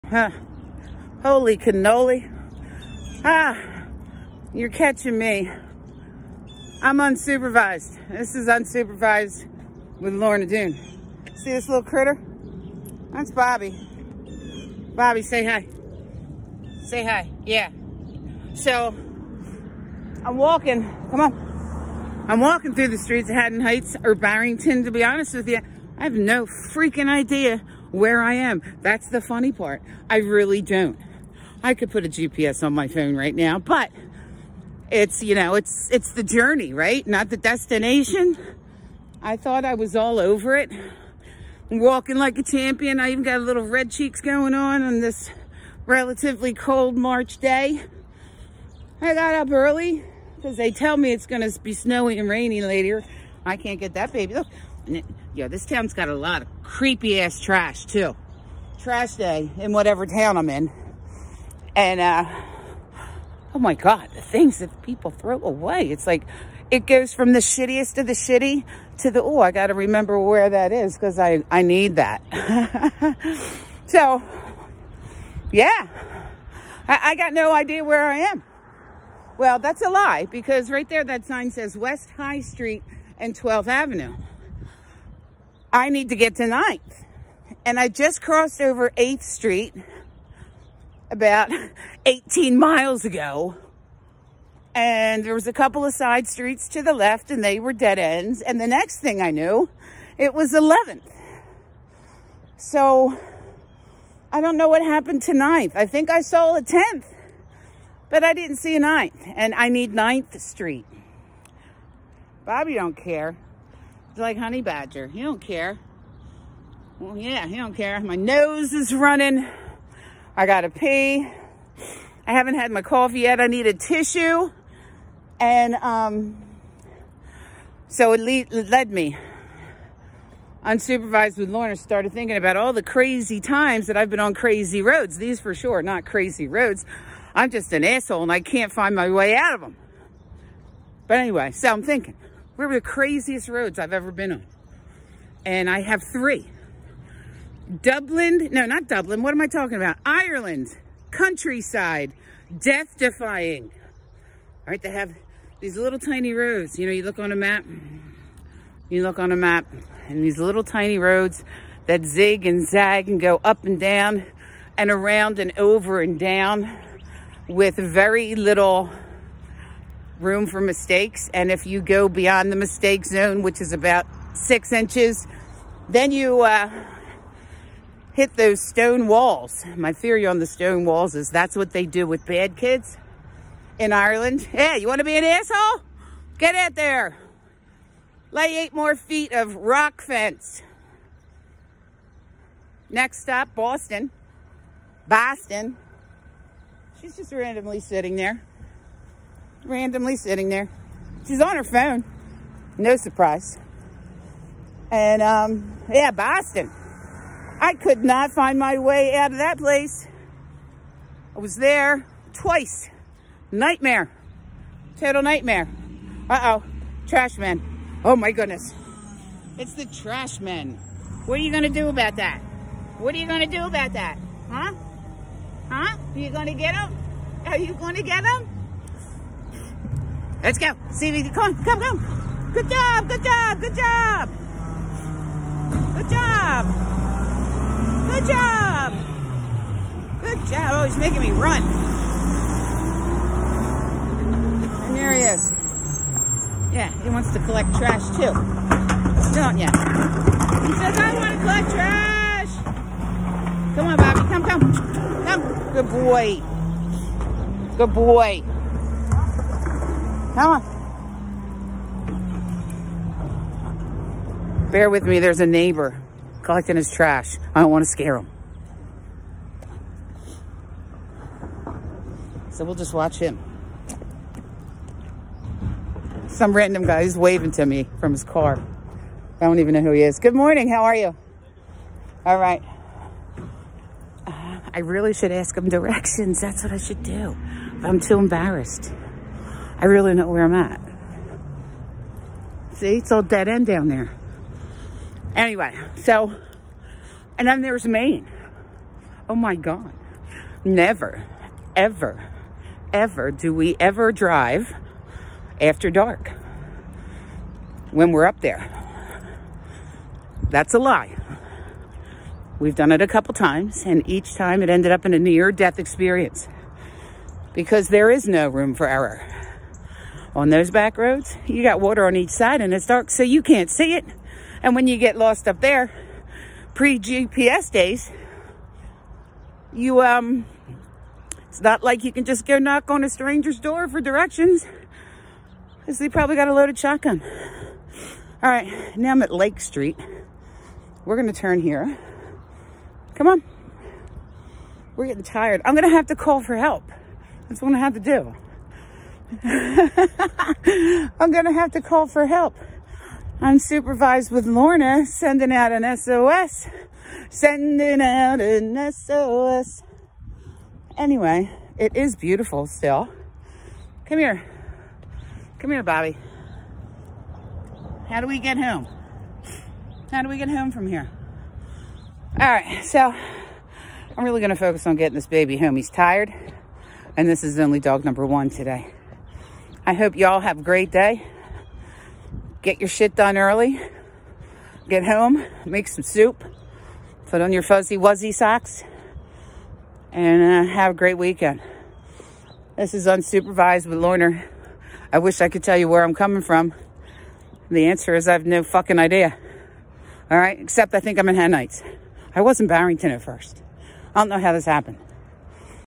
Lost in the hood with Lorna
How to train a dog To get you back home when you have lost your way.